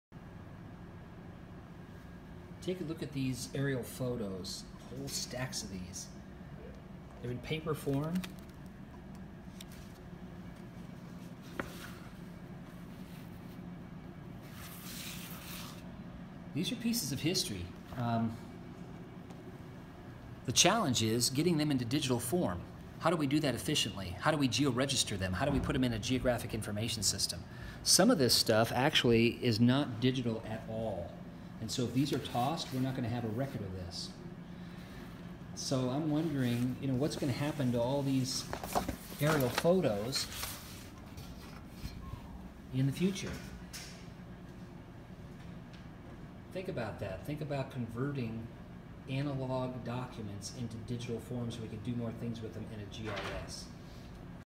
Converting analog aerial photos into digital form
Converting analog aerial photos into digital form:  Considerations.  Much spatial data has been converted into digital form, but much has not.  Who will do it?  Do we need to do it?